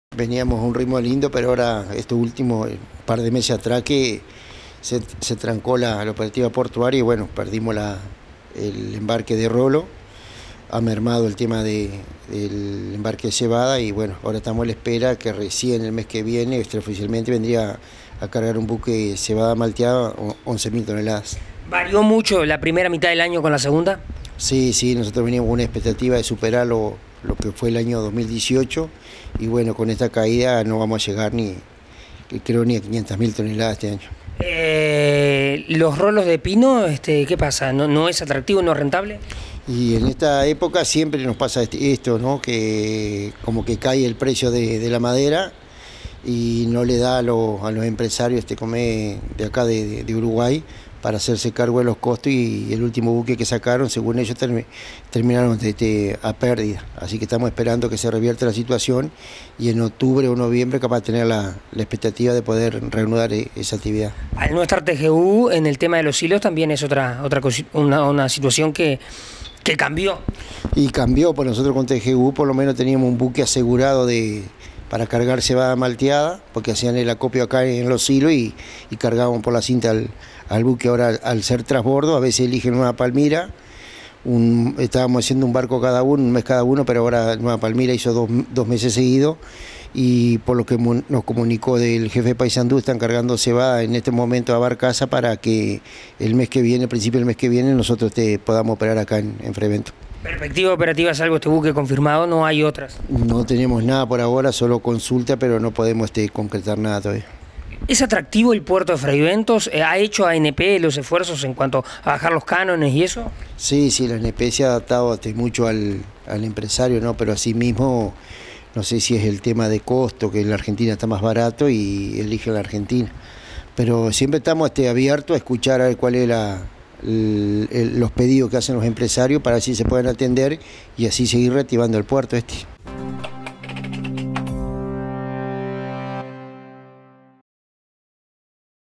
Bajó el movimiento portuario en el segundo semestre del año.
Julio Rodriguez integrante de la administración del puerto de Fray Bentos confirmó que bajó considerablemente el movimiento portuario en el segundo semestre del año.